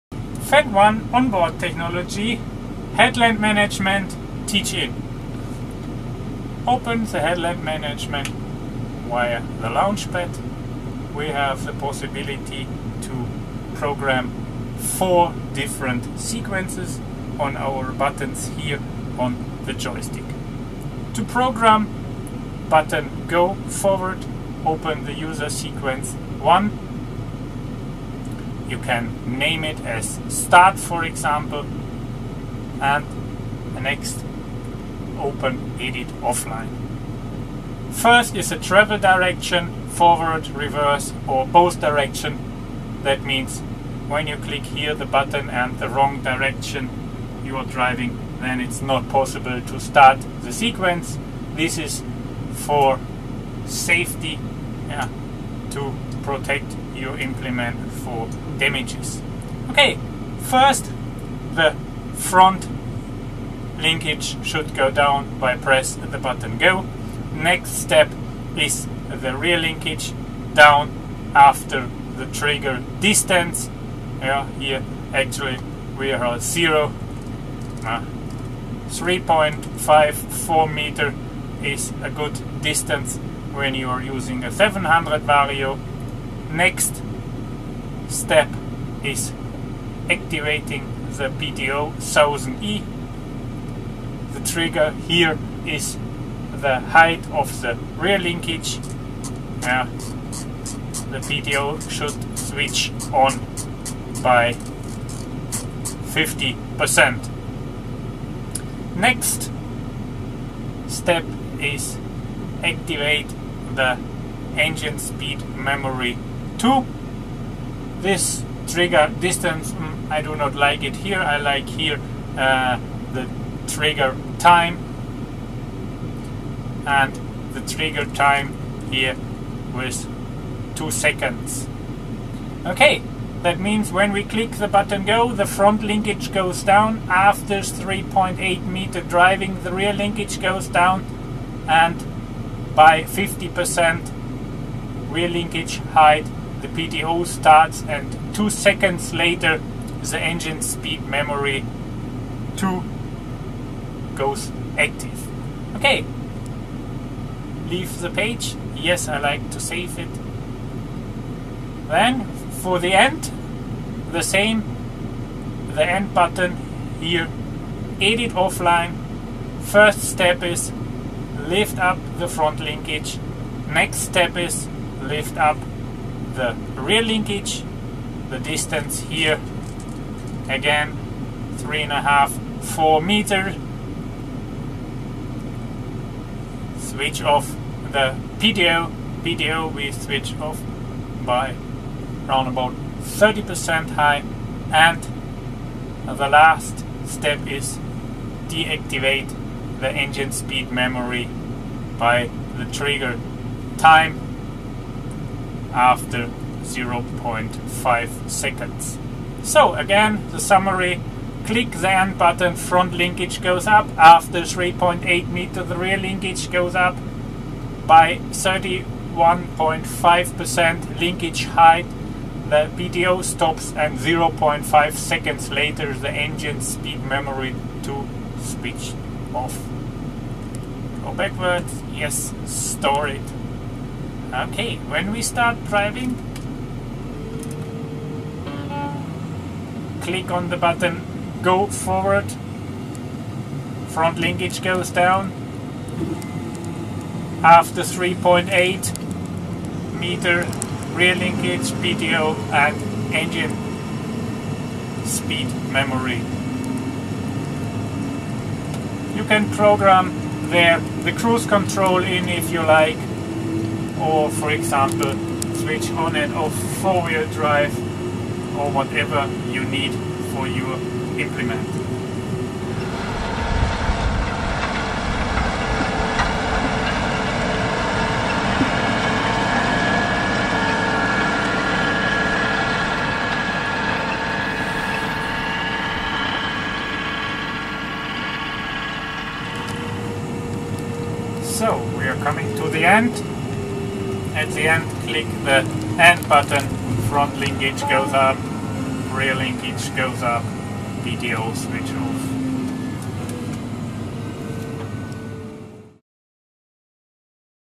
How-to | FendtONE onboard | Part 30 | Headland Management partical application | Fendt
30 Headland management in practical use - This video explains how to store headland sequences of the headland management TeachIn on the four keys of the travel lever. These are then demonstrated in practical use.